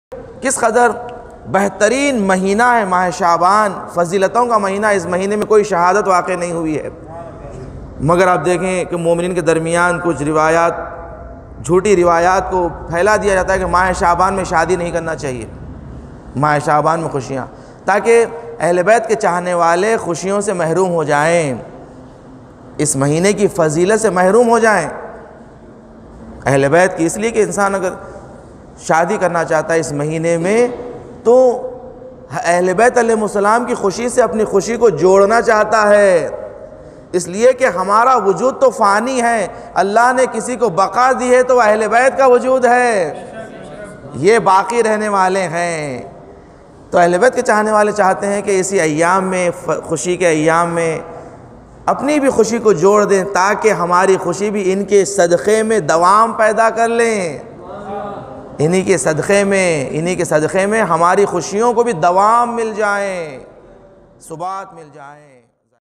Mah Shaban Mai Shadi/Nikah Karna Kaise hai..?
Jaiz Ya Makrooh..

Kiya Shaban ke mahine mai Shadi kar sakte hai..?

Maulana Syed Hannan Razvi Sahab Qibla